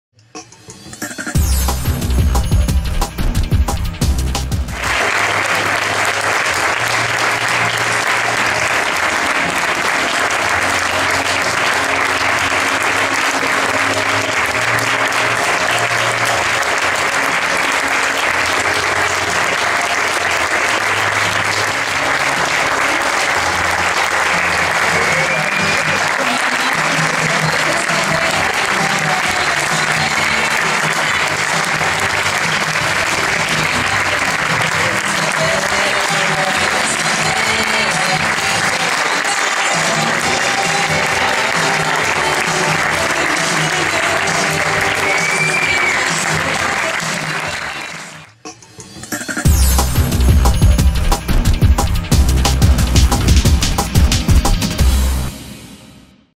"BAGNATI" di Tobia Rossi - Applausi finali al cast - MILANO, TEATRO LIBERO, 13.6.2015
Loro sono ormai un consolidato duo artistico. Tanto che, dal 3 al 15 giugno 2015 il drammaturgo Tobia Rossi e il regista Manuel Renga porteranno, in prima nazionale, al Teatro Libero di Milano la loro nuova commedia, “Bagnati”, che parlerà di sessualità e scoperta di sé. Già, perché sul palco s’indagheranno le perversioni dei figli della “Milano bene”, il pissing, proprio nell’estate dell’Expo, con attori di talento come Alex Cendron, Paola Giacometti, Federico Lotteri, Giulia Merelli, Claudia Negrin e Daniele Pennati. Immagini dalla recita del 13 giungo 2015.